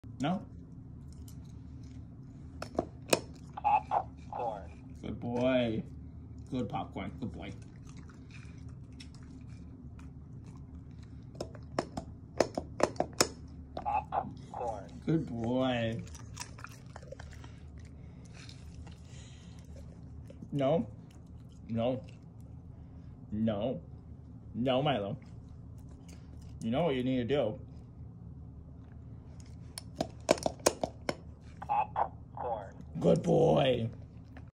Corgi pushes button to talk
Milo Reginald pushes his Popcorn button to tell dad he wants more popcorn!!!